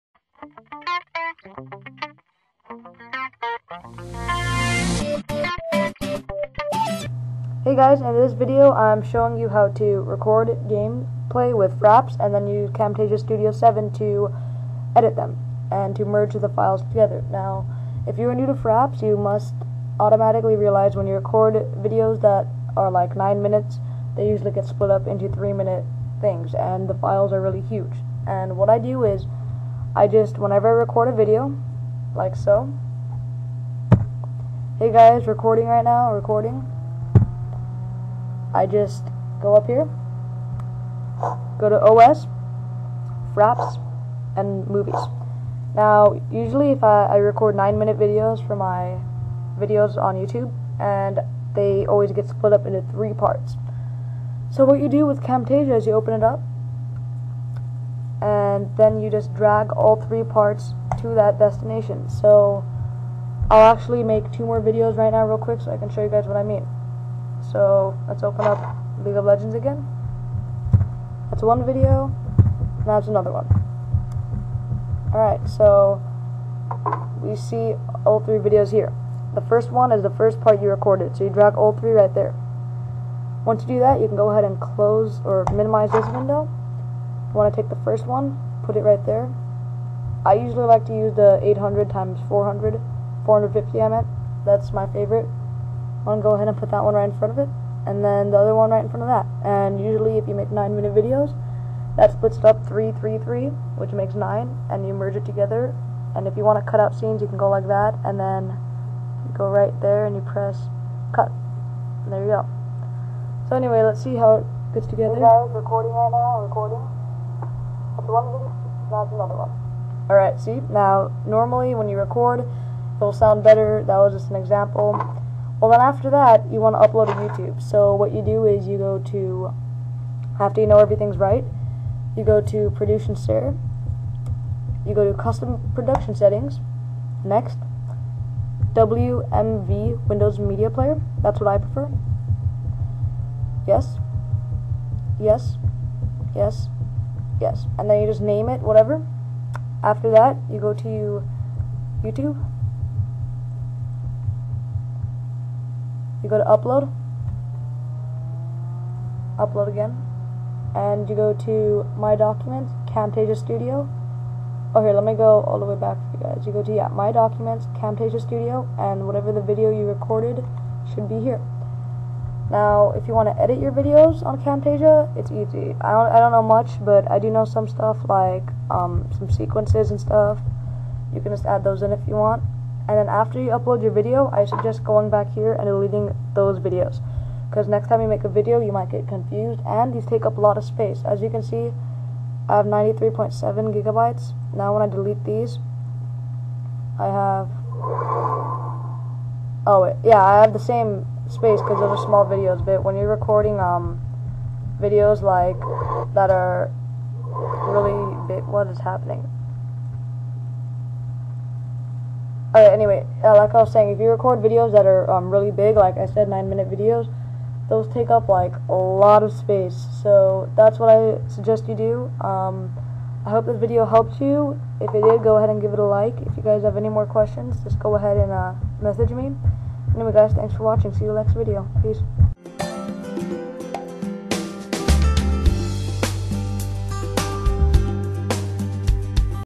How To Edit FRAPS Videos With Camtasia Studio 7 + Upload And Merge Them
Hey guys, the tittle says it all. Enjoy! =]